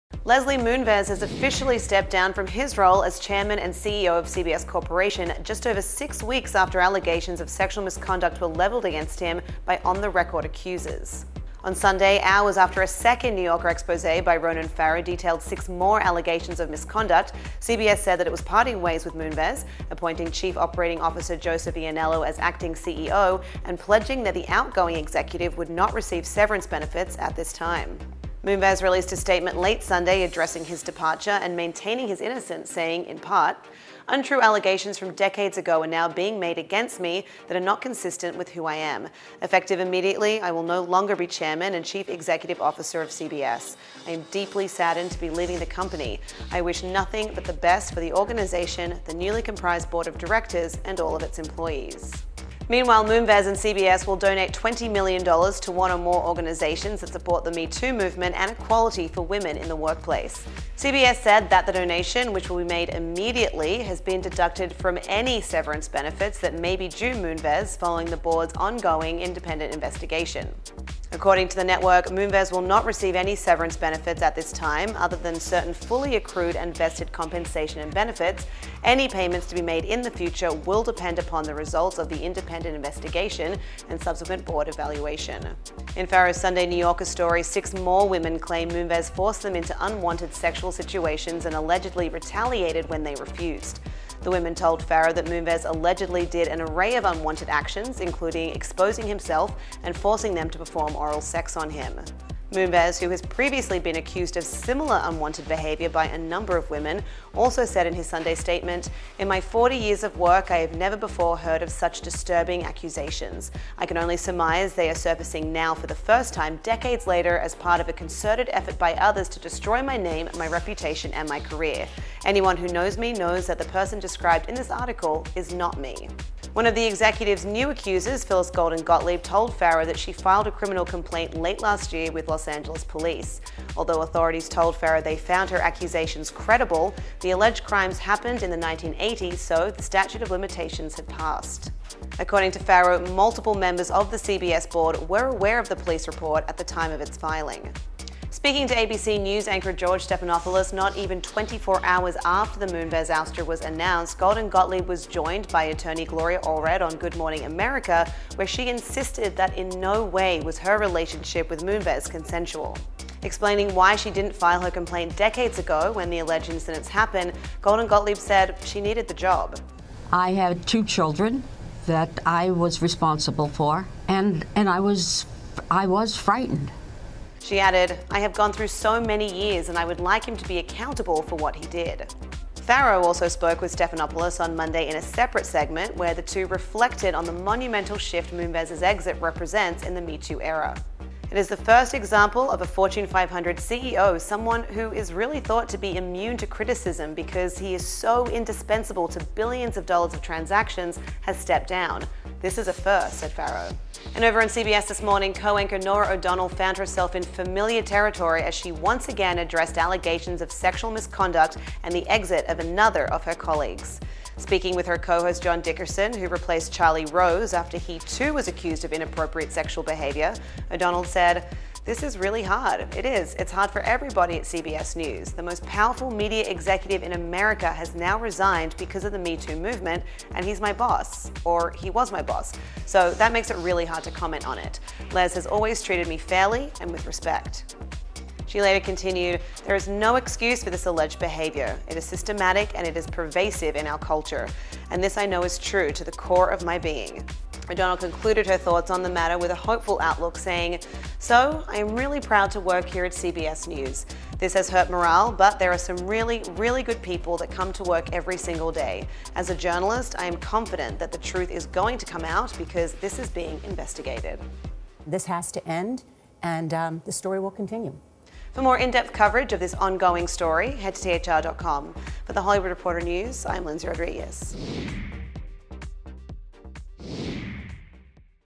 Leslie Moonves Accuser Says His Response Is a "Joke" | THR News
Leslie Moonves has officially stepped down from his role as chairman and CEO of CBS Corp, just over six weeks after allegations of sexual misconduct were leveled against him by on the record accusers.